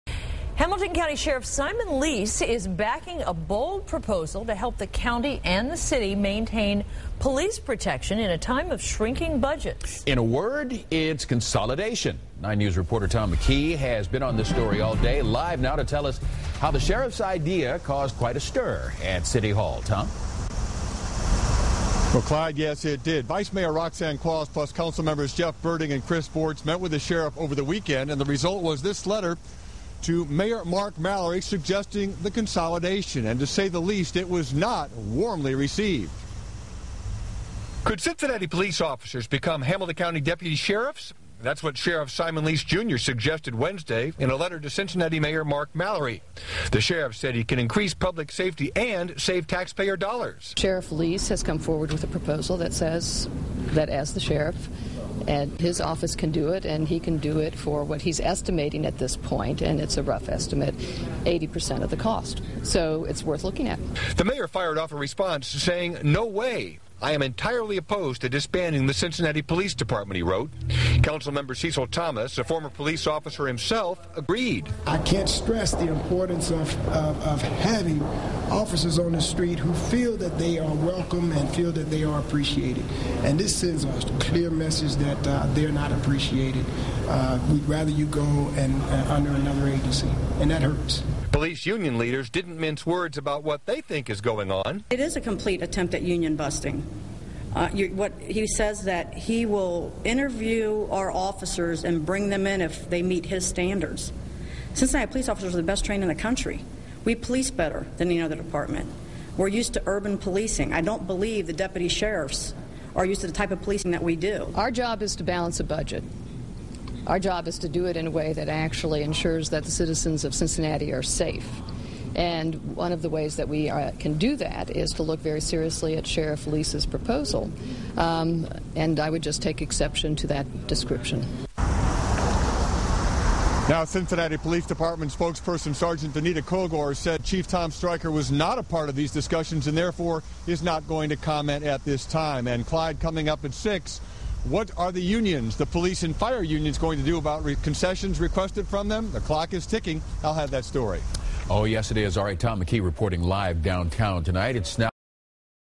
Sheriff backs proposal to merge city, county police
Hamilton County Sheriff Simon Leis is backing a bold proposal to help the county and city maintain police protection in a time of shrinking budgets.